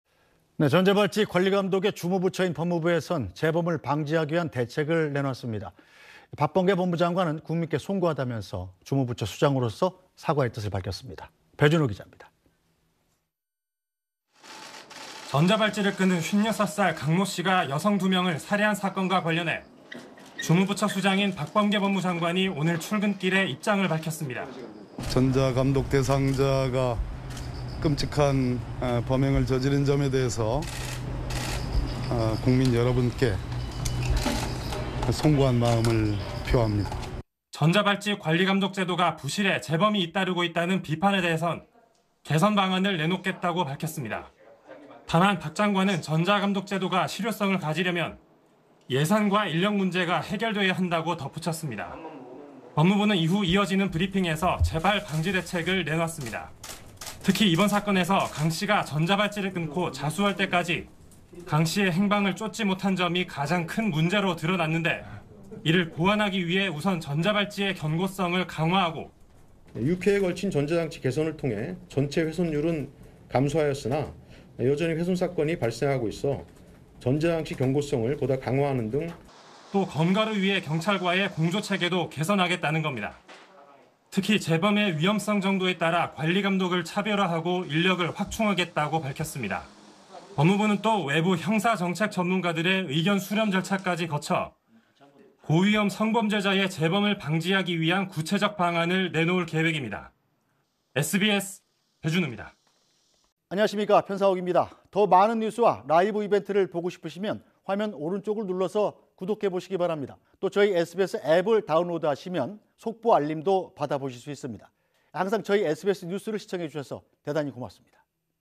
박범계 "전자감독 대상자 끔찍한 범행…국민께 송구" / SBS
전자발찌 관리감독의 주무부처인 법무부에서는 재범을 방지하기 위한 대책을 내놨습니다 박범계 법무부 장관은 국민께 송구하다면서, 주무부처 수장으로서 사과의 뜻을 밝혔습니다.
배준우 기자입니다.